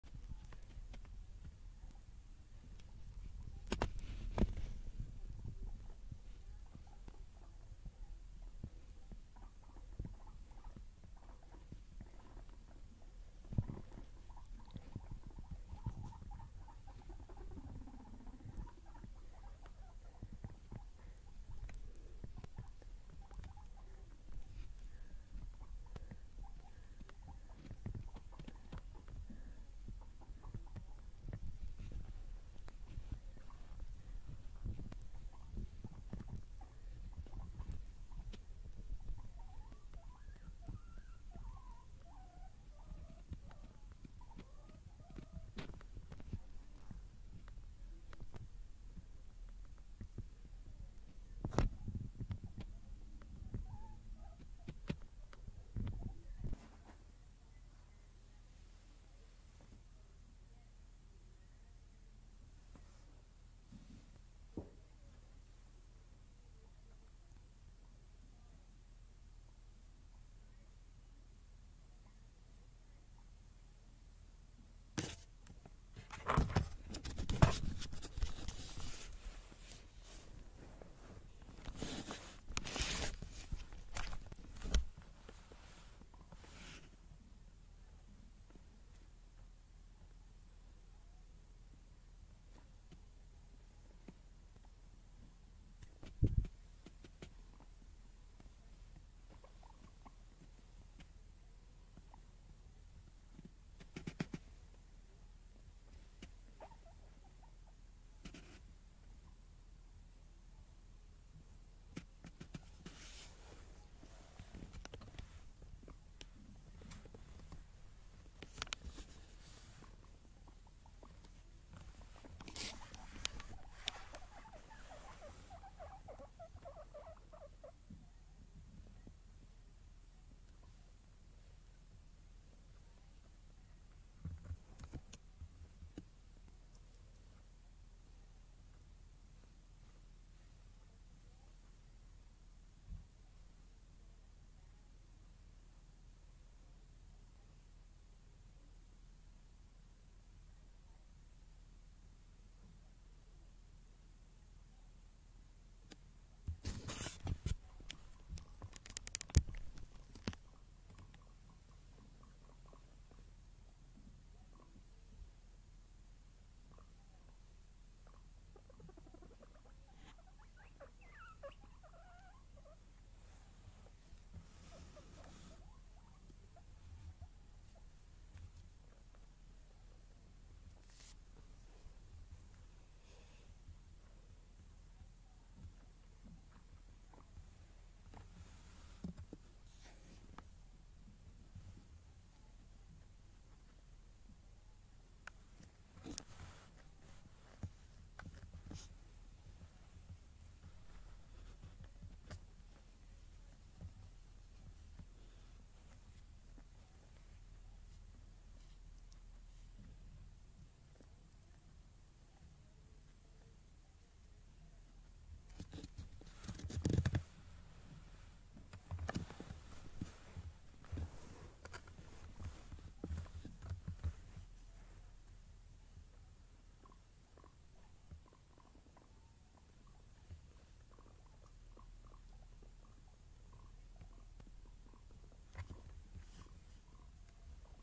Guinea pigs walking under blanket
Just some lumps moving, making noise. 😂